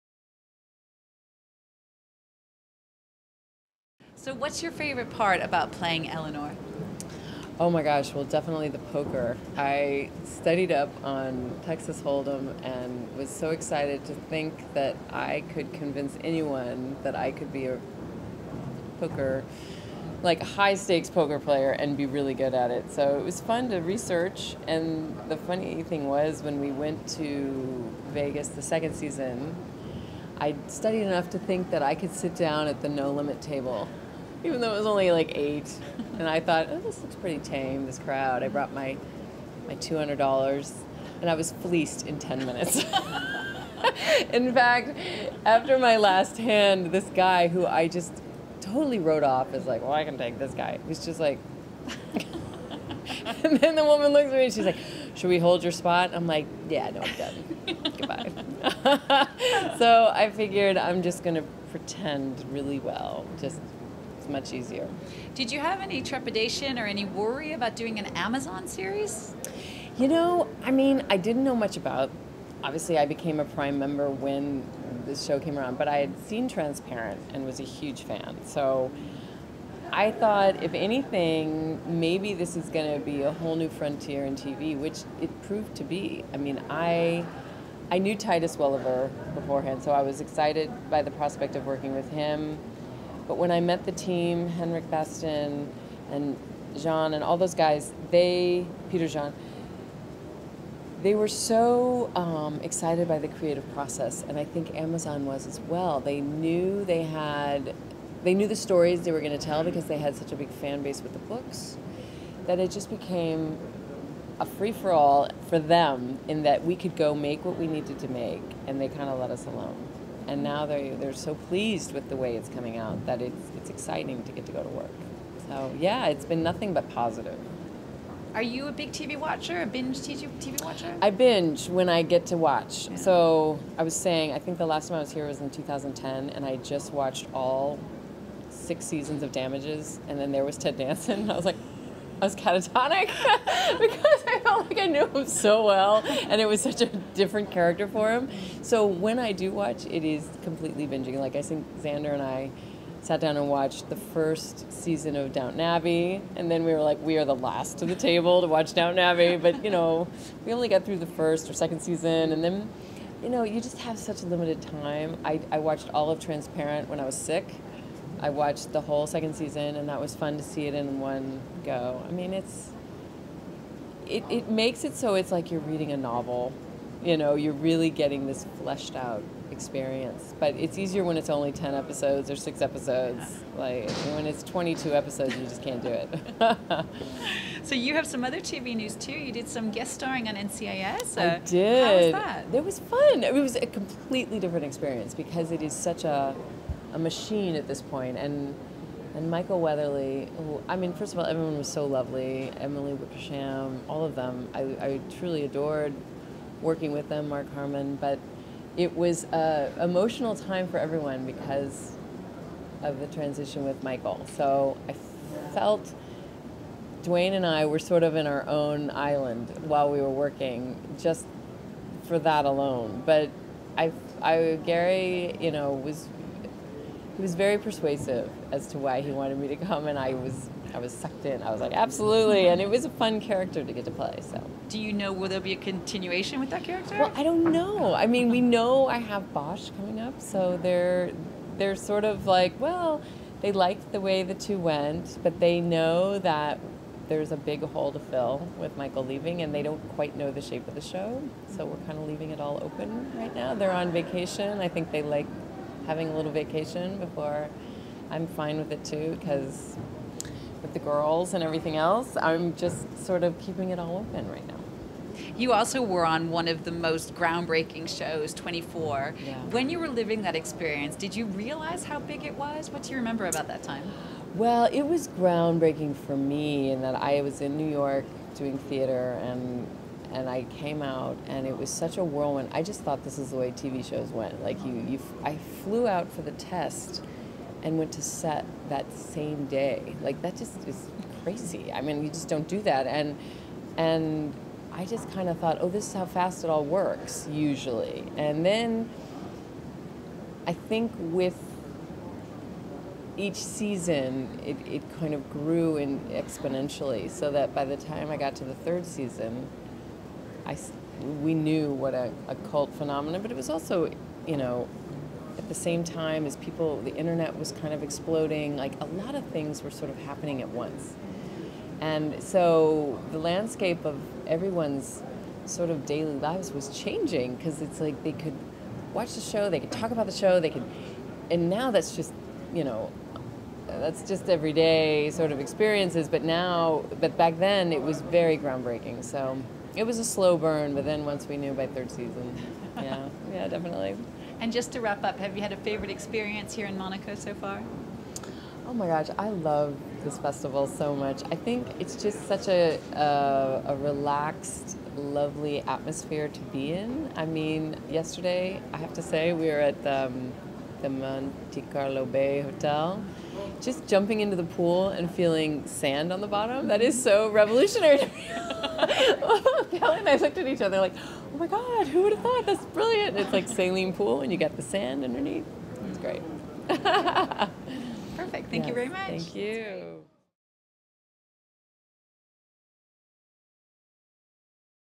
Sarah Clarke BOSCH | FTV16
Interview with Sarah Clarke, Bosch, at 2016 Monte-Carlo Television Festival